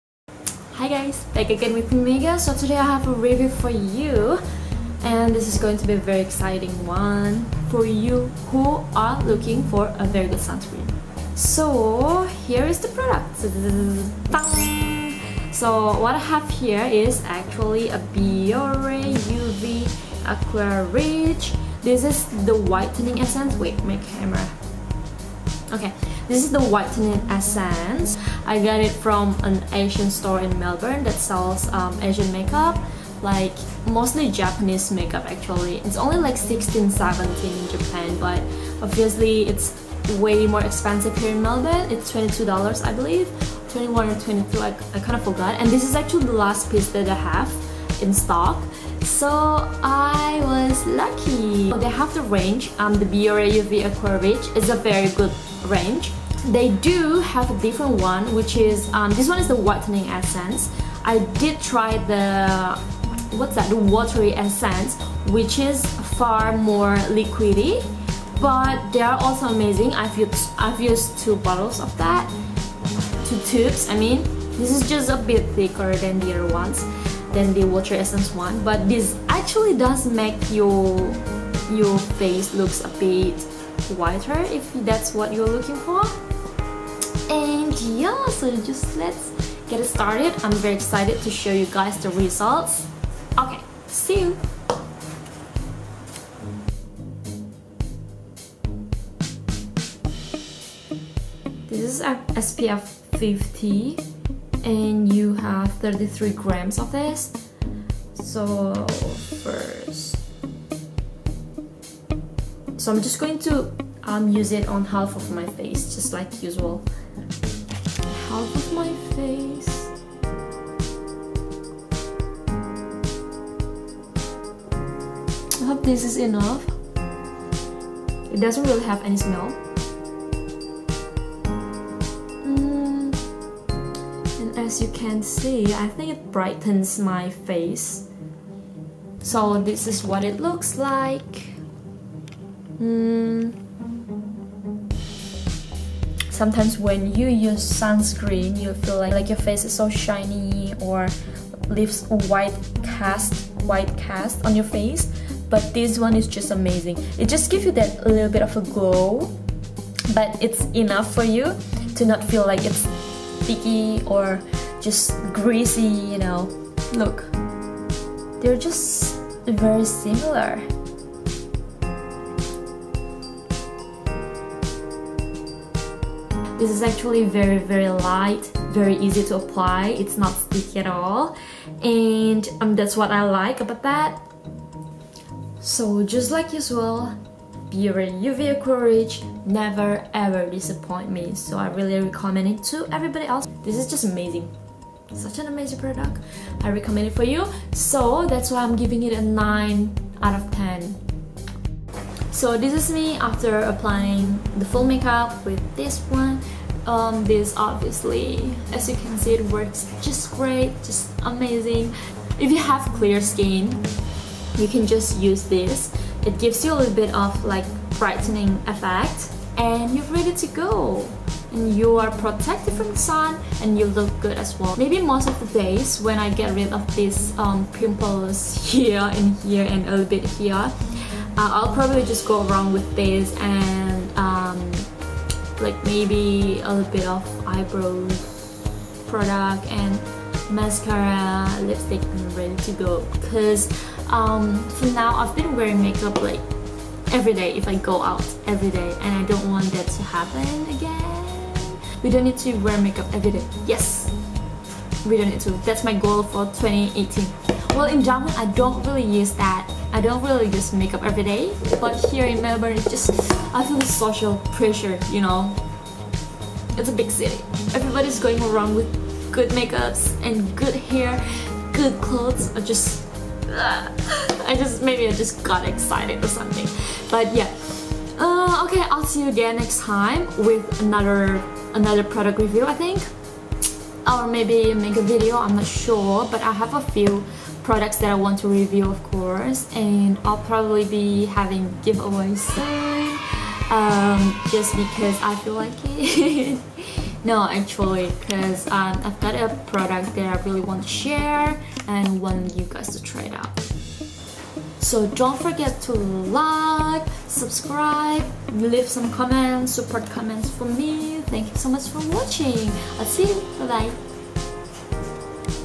[REVIEW] BIORE UV Aqua Rich Whitening Essence (INDONESIAN SUB)
Holla, I'm back back back! In this video, I'll give a review about a product that I've used for months which is BIORE UV Aqua Rich Whitening Essence. This is a perfect sunscreen for people who hates sticky faces after applying sunscreen (ps: this also give you whitening effect without looking cakey)

HOW AMAZING IS THAT? Check out the video for more explanation :))

Yours sincerely,

Megga